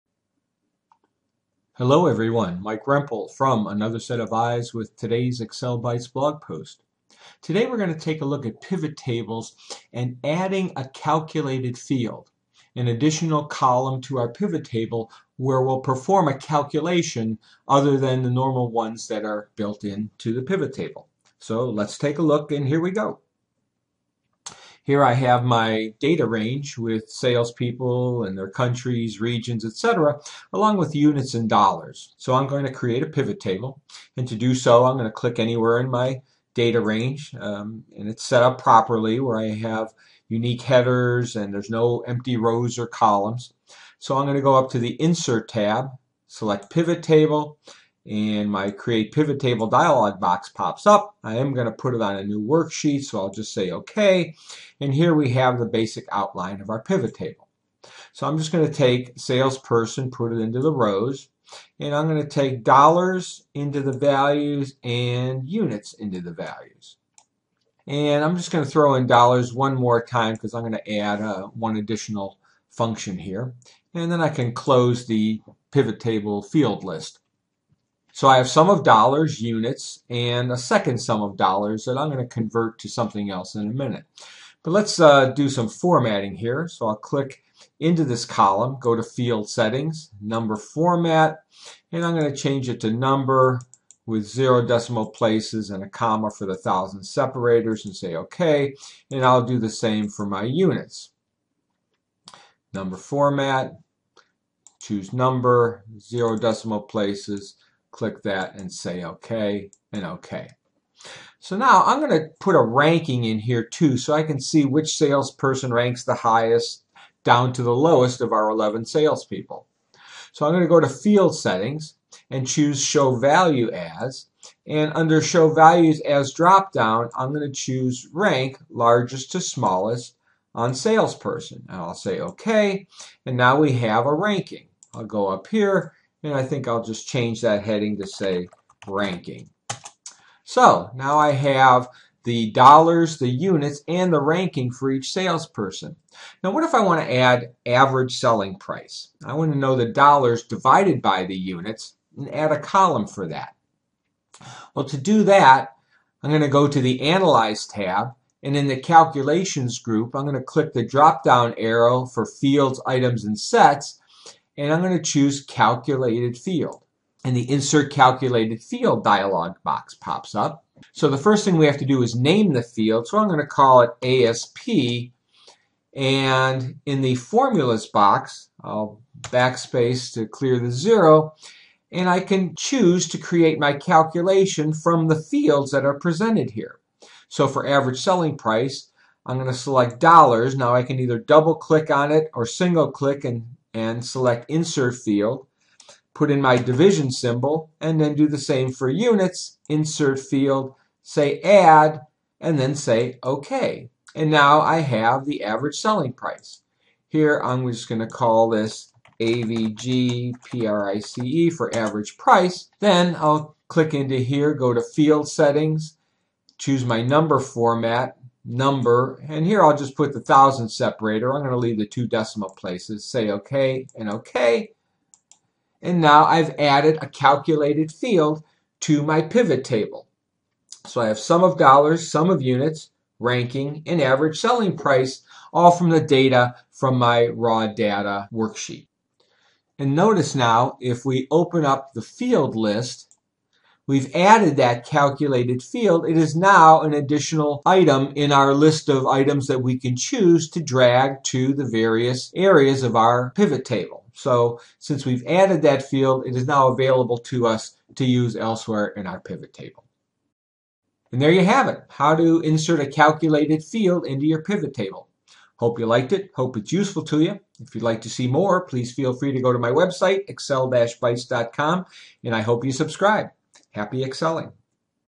How To Add A Calculated Field In A PivotTable In Excel
Here is how you can insert an additional field into a PivotTable to perform a calculation.  You can download the file here and follow along.  If you get a preview, look for the download arrow in the upper right hand corner.